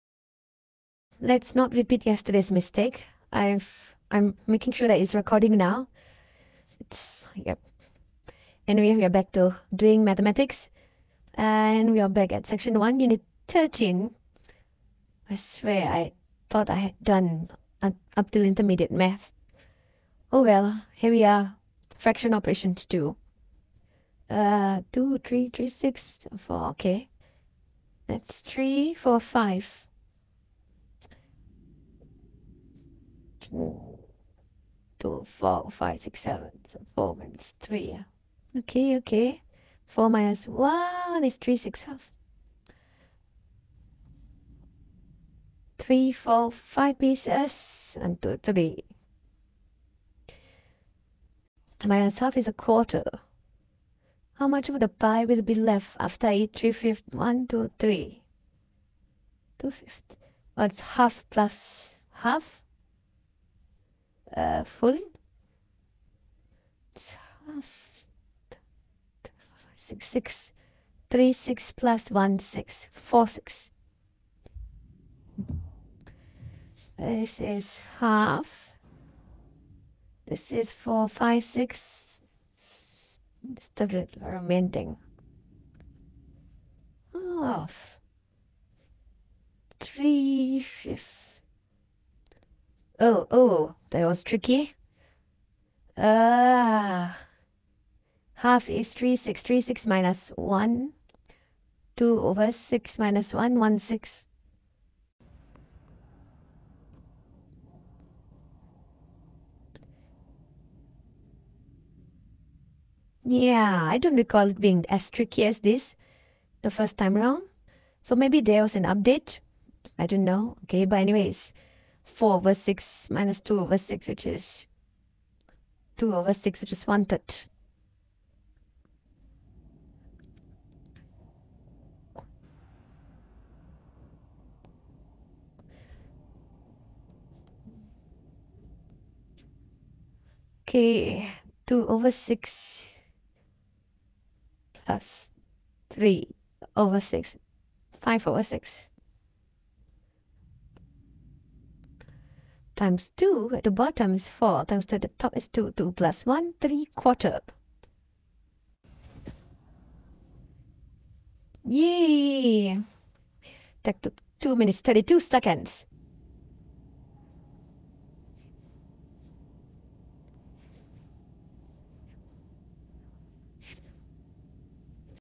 #1818 Duolingo Math | Fraction Operations 2
Ready to conquer fractions? In this Duolingo Math lesson on Fraction Operation 2, we break down how to add, subtract, multiply, and divide fractions like a pro. Whether you’re brushing up for school, homeschooling, or just love learning, this step-by-step walkthrough will help you master the math fast! Perfect for learners of all ages—join the streak and level up your math skills today!